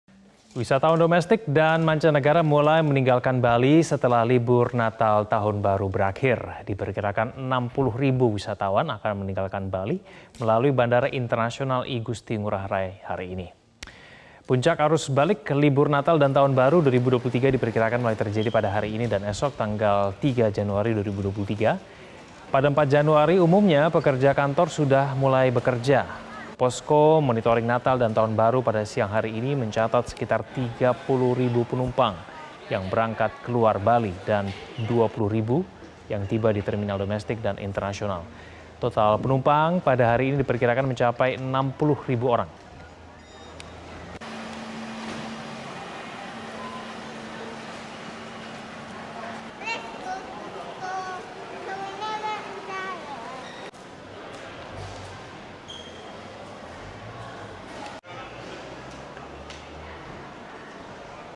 Arus Balik Libur Nataru, 60 Ribu Wisatawan akan Tinggalkan Bali
MetroTV, Wisatawan domesik dan mancanegara mulai meninggalkan Bali, setelah libur Natal tahun baru, berakhir. Diperkirakan 60 ribu wisatawan akan meninggalkan Bali, melalui Bandara Internasional I Gusti Ngurah Rai, pada Senin (2/1/2023).

Puncak arus balik libur Natal dan tahun baru 2023 diperkirakan mulai terjadi pada Senin dan esok tanggal 3 januari 2023. Pada 4 januari umumnya pekerja kantor sudah mulai kerja.

Posko monitoring Natal dan tahun baru pada Senin (2/1/2022) siang ini, mencatat sekitar 30 ribu penumpang yang berangkat keluar Bali dan 20 ribu yang tiba di terminal domestik dan internasional.  Total penumpang hari ini diperkirakan mencapai 60 ribu orang.